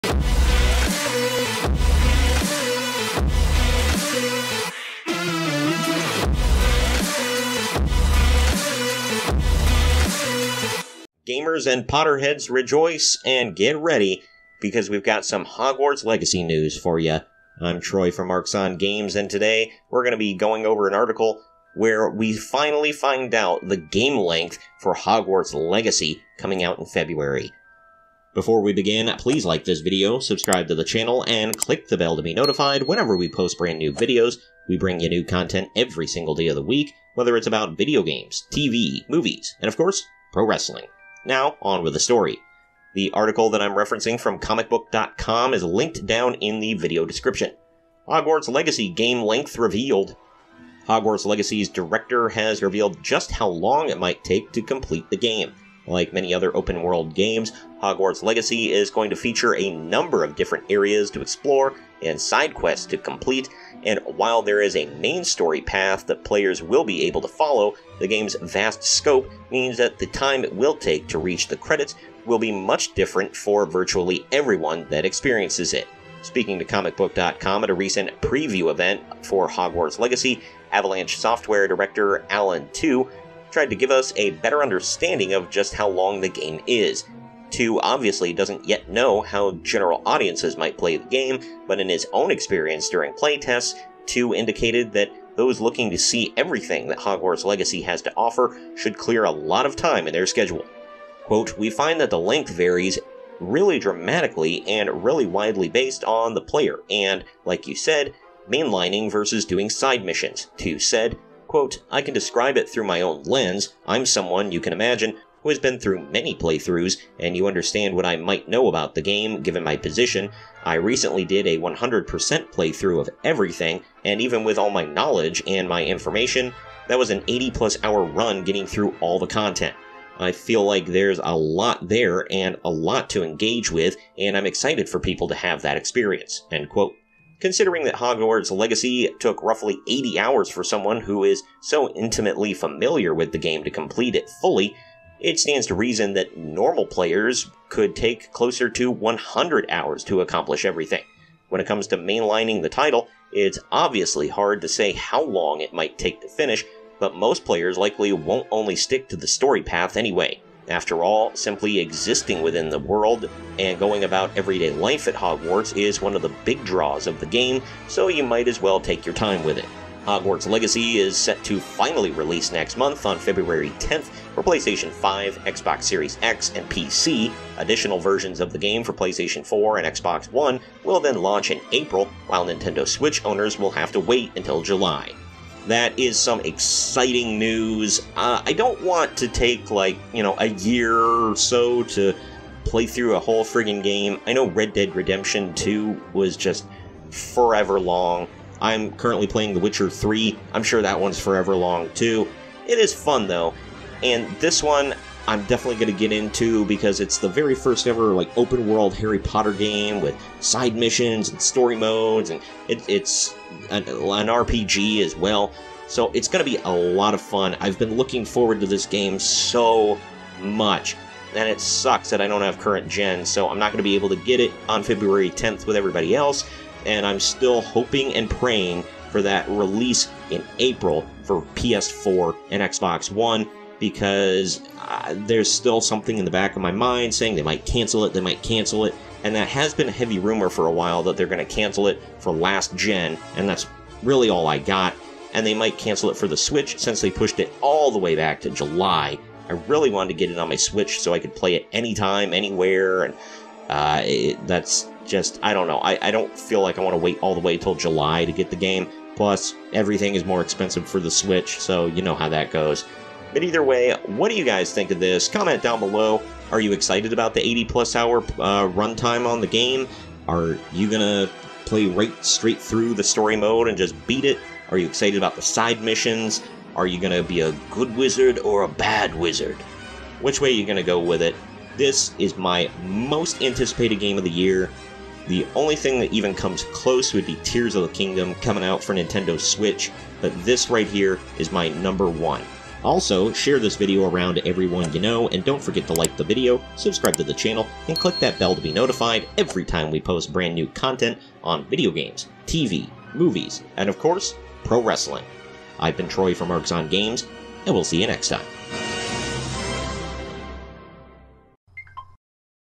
Hogwarts Legacy Game Length Will Be Huge
Those looking to purchase the new Hogwarts Legacy video game better clear their schedules. This game is gonna take quite a bit of time to complete!

LISTEN EVERY WEDNESDAY AT 9PM EST